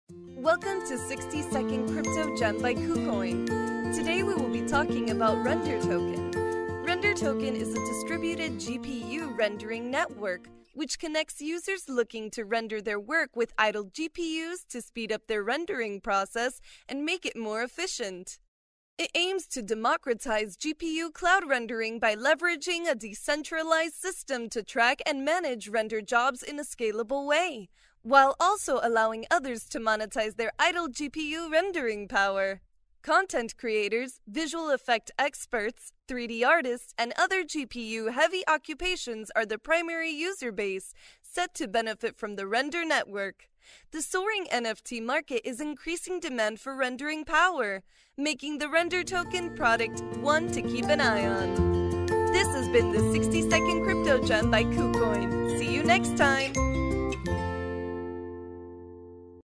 KuCoin 60s Crypto Gem | Render Token (RNDR): Distributed GPU Rendering on the Blockchain
Render Token is a distributed GPU rendering network, which connects users looking to render their work with idle GPUs to speed up the rendering process and make it more efficient. It aims to democratize GPU cloud rendering by leveraging a decentralized system to track and manage render jobs in a scalable way, while also allowing others to monetize their idle GPU rendering power. Content creators, visual effect experts, 3D artists, and other GPU-heavy occupations are the primary user base set to benefit from the Render Network. The soaring NFT market is increasing demand for rendering power, making the Render Token product one to keep an eye on.